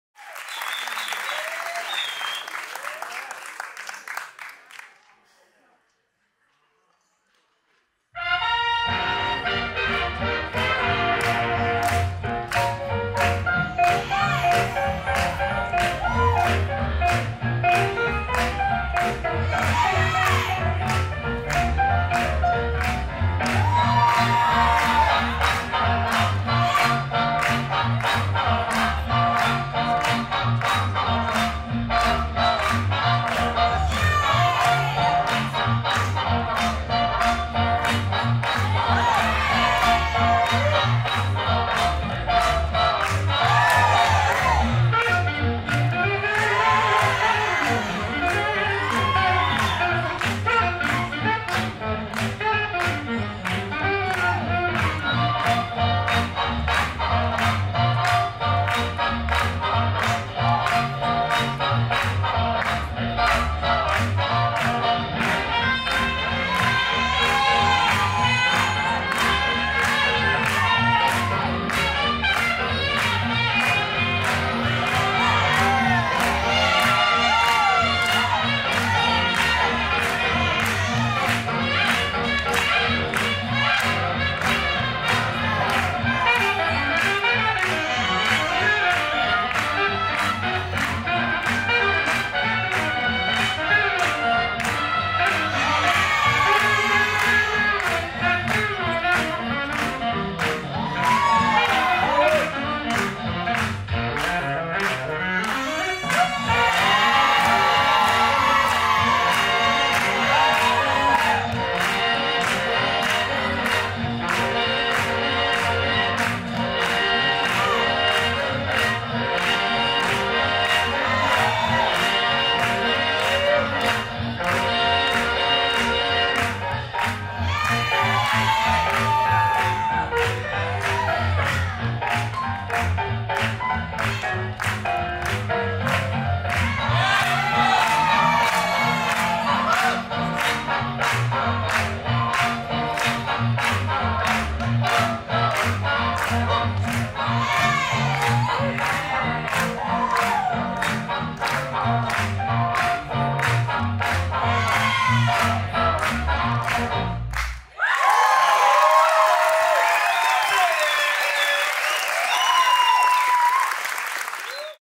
Marcos Leon & Roser Ros Masclans - All-Star Classic Routine - ILHC EUROPE 2022
The International Lindy Hop Championships Europe takes place, in person, each year in Sofia, Bulgaria. for more information.

All-Star Classic Routine - Marcos Leon & Roser Ros Masclans (Spain)

ILHC Europe 2022 Video Team:
Nuno Neves, Lina Martinkenaite-Bendike, Emiliya Bazlyankova, Richard Chuks

Powered by: ILHC, Lindy Hop Bulgaria, Remy Kouakou Kouame.

This event is part of the project Swinging Europe Network. Co-funded by the Creative Europe Programme of the European Commission.